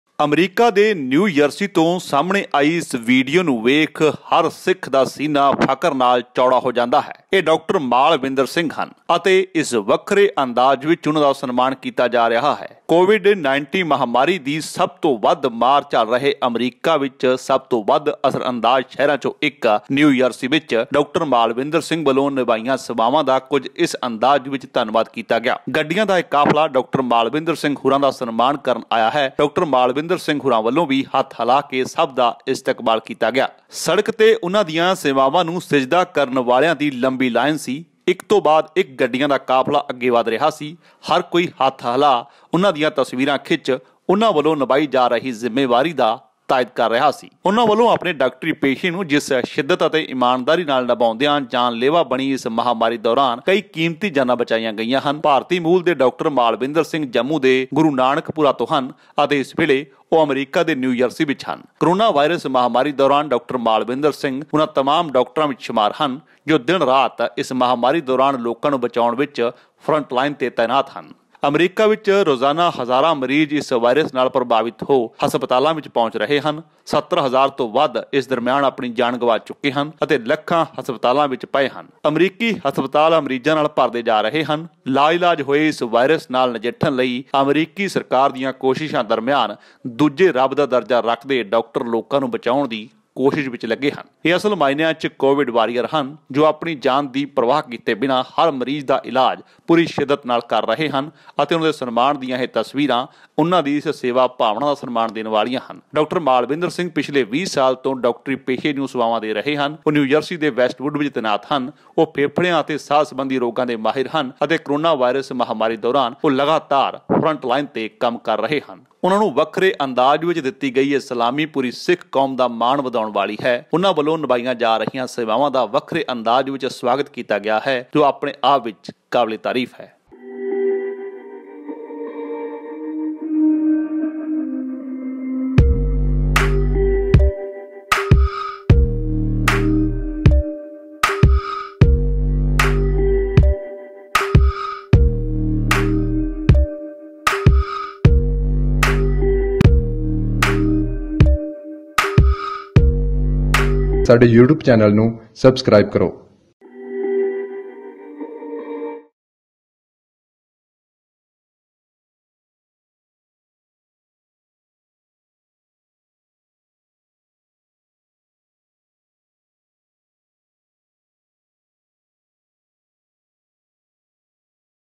USA ਦੇ SIKH DOCTOR MALVINDER SINGH ਦਾ ਗੱਡੀਆਂ ਦੇ ਕਾਫਿਲੇ ਨਾਲ ਵੱਖਰੇ ਅੰਦਾਜ ‘ਚ ਸਨਮਾਨ || SNE
USA ਦੇ SIKH DOCTOR MALVINDER SINGH ਦਾ ਗੱਡੀਆਂ ਦੇ ਕਾਫਿਲੇ ਨਾਲ ਵੱਖਰੇ ਅੰਦਾਜ ‘ਚ ਸਨਮਾਨ || SIKH TELEGRAPH ||SNE